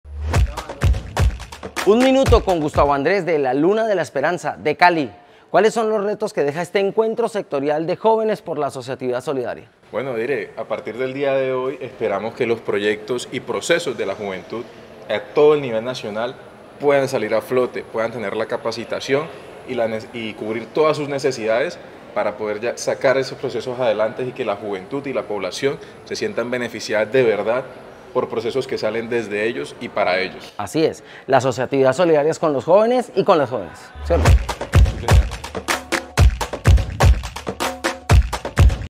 Un Minuto con Gustavo Andrés - Fundación Luna de la Esperanza
En el Encuentro Sectorial de Jóvenes en Buenaventura nuestro director
Mauricio Rodríguez Amaya, habló con Gustavo Andrés, de la Fundación Luna de la Esperanza que, En1Minuto le contó cuáles son lo retos que quedaron luego de este importante evento para la juventud del Valle del Cauca.